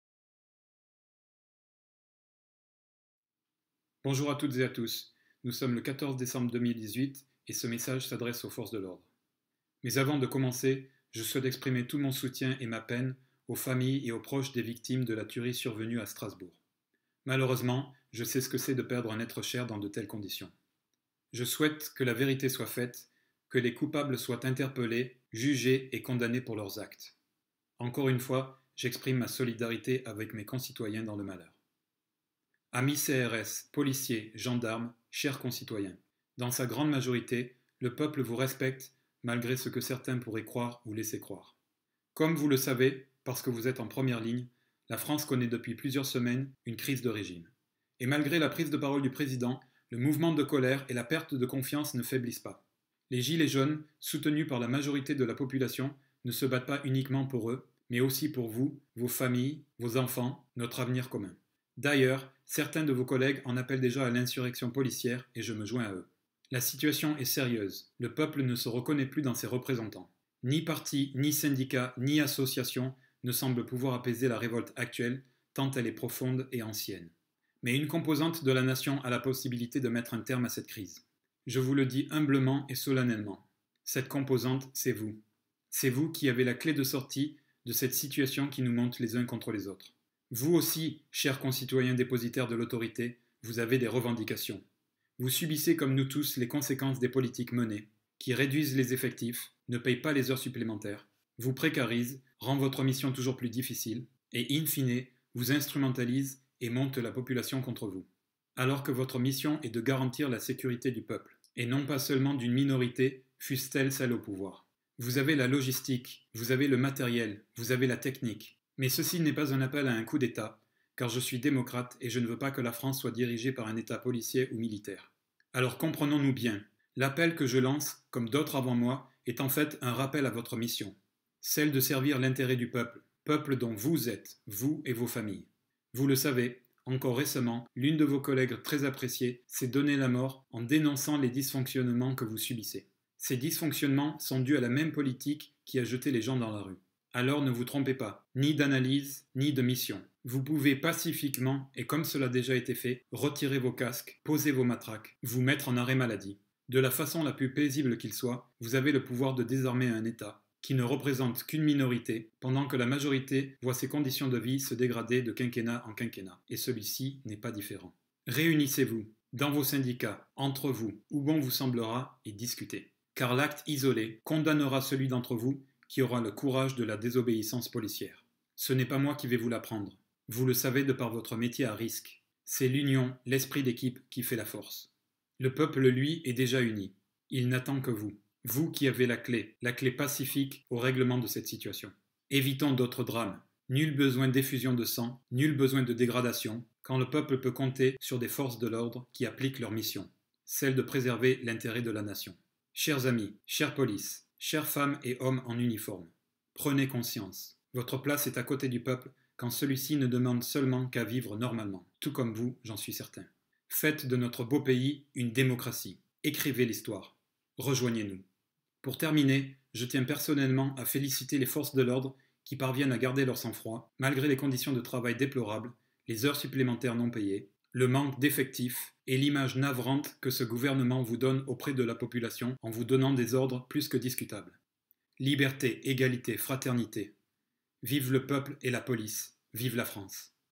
Appel aux Forces de l'Ordre
Ce sont nos concitoyens représentants des force de l'ordre qui ont la clé du dénouement pacifique de la crise politique qui s'incarne dans le mouvement des gilets jaunes.

Ceci et donc un appel, humble et solennel, à nos concitoyens policiers, CRS, gendarmes.

Écrivez l'histoire. Faite de notre beau pays une démocratie. Rejoignez le peuple en déposant casques et matraques.

L'union fait la force. La force du peuple. Peuple dont vous êtes.

Liberté, égalité, fraternité. Vive la France !